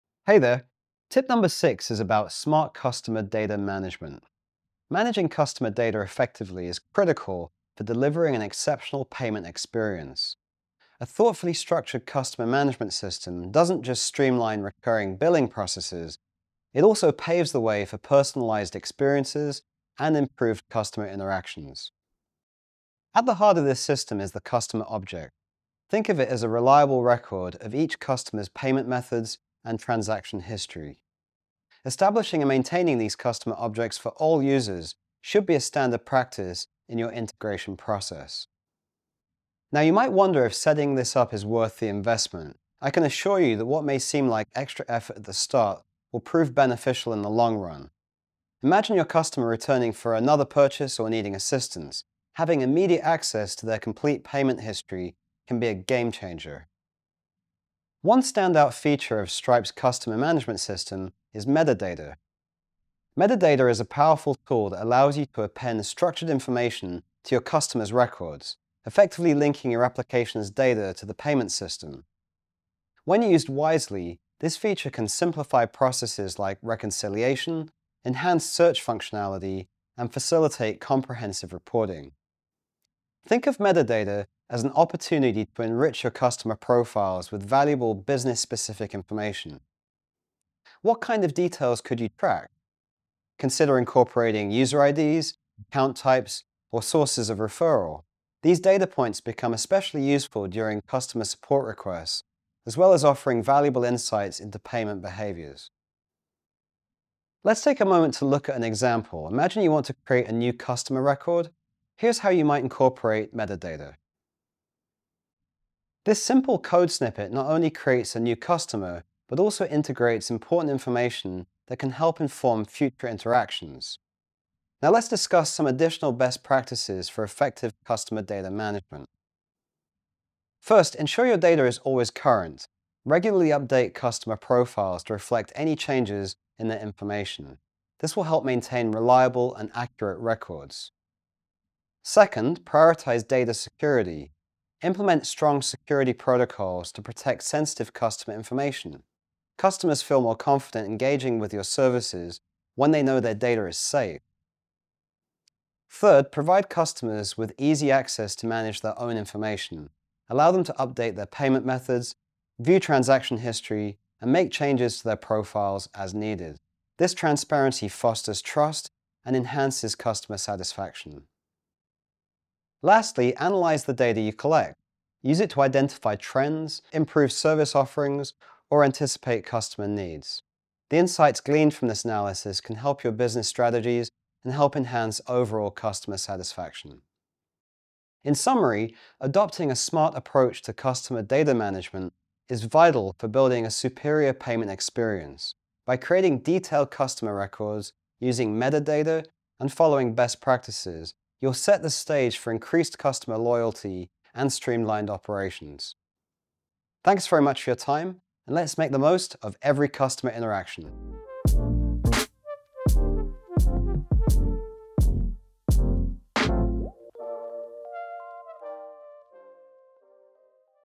Implement smart customer data management – Building rock-solid Stripe integrations #6
Unlock the secrets to delivering exceptional payment experiences through effective customer data management! In this video, we explore the importance of maintaining accurate customer records, enhancing your system with metadata, and implementing best practices for handling customer data.

Join us as we dive into topics like:
- The significance of Customer Objects in payment processing
- How metadata can streamline reconciliation and reporting
- Best practices for keeping customer information up-to-date and secure
- Real-world code examples to enhance your integration

Whether you're a business owner, developer, or customer support professional, you'll find valuable insights to improve customer interactions and boost satisfaction.

### Presenter
James Beswick - Developer Advocate